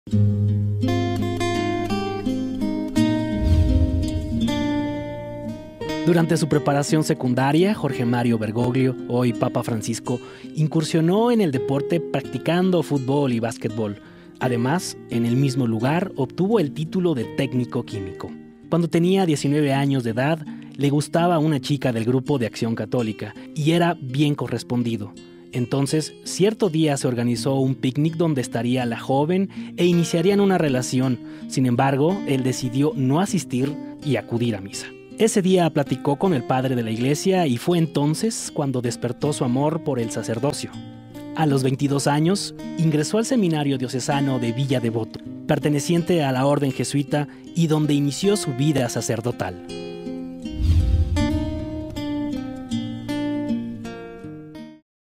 Cápsula 2 Juventud
Durante su preparación secundaria Jorge Mario Bergoglio, hoy Papa Francisco, incursionó en el deporte practicando futbol y basquetbol.
Además, en el mismo lugar, obtuvo el título de técnico Químico.